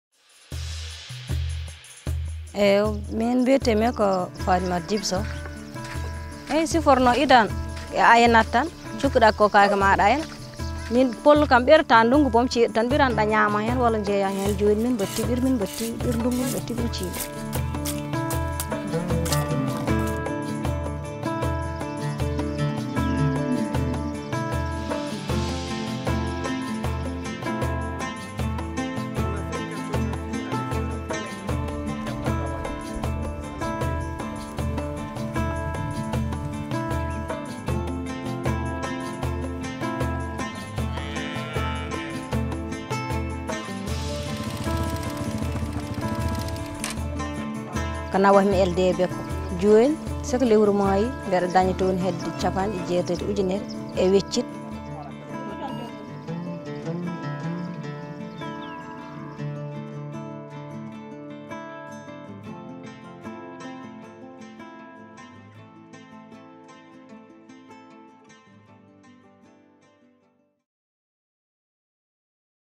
Improving pastoral yields for more local milk
Today in Africa, the demand for dairy products is increasing. However, dairies have difficulties in sourcing local milk in terms of volume, regularity and quality. The Africa-Milk project supports the co-design and implementation of technical, organisational and institutional innovations to increase and secure local milk supply in Senegal, Madagascar, Burkina Faso and Kenya.